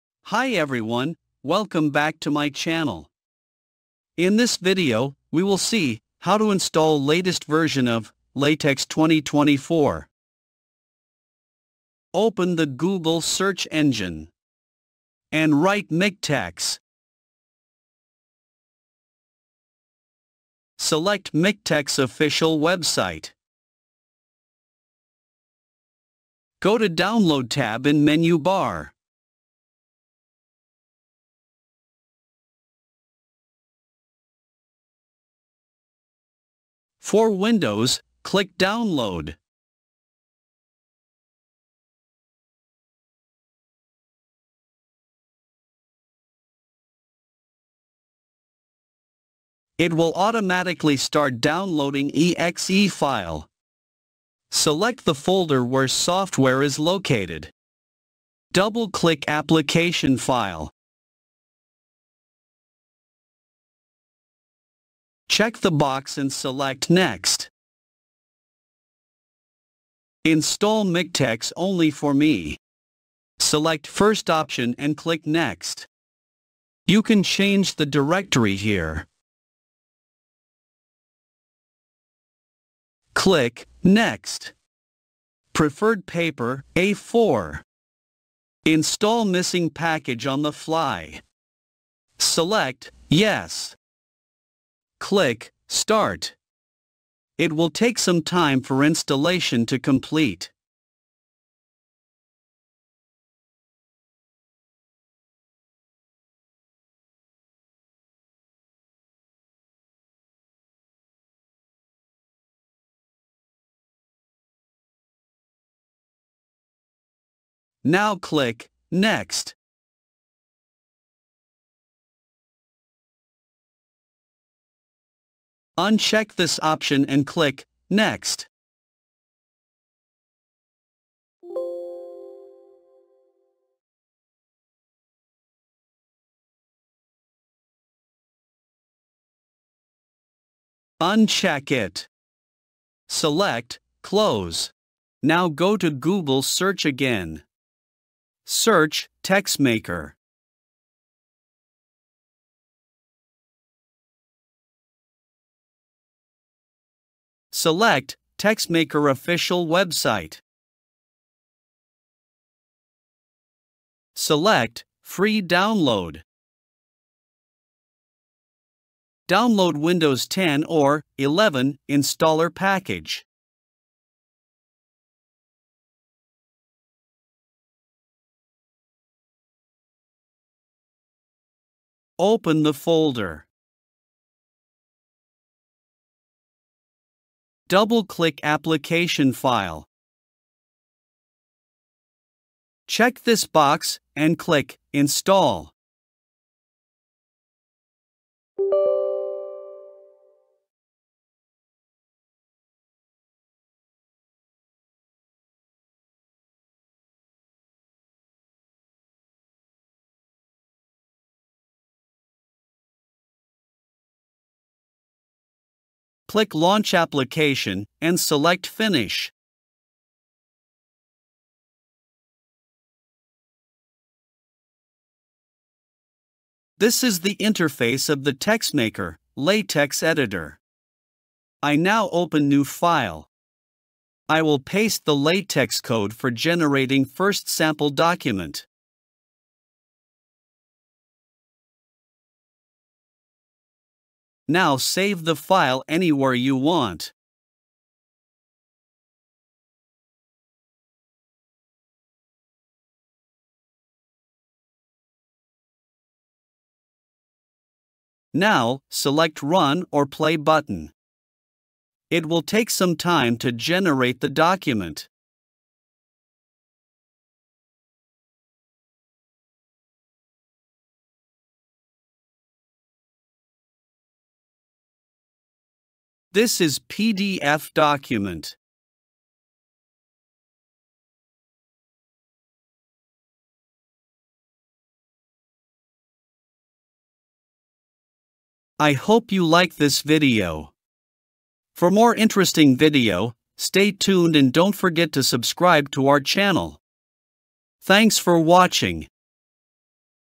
Install LaTeX 2024 | MikTEX & TEXmaker step by step Installation
How to Install LaTeX 2024 | MikTEX & TEXmaker Installation

In this video, I provide a step-by-step guide for installing LaTeX. I demonstrate the installation of the latest LaTeX system (2024) on MS Windows 10 & 11. The process includes downloading and installing the full LaTeX distribution, MiKTeX, followed by the installation of TeXmaker which is most popular latex editor.

Don't forget to **LIKE** and **COMMENT** – I’d love to hear your feedback!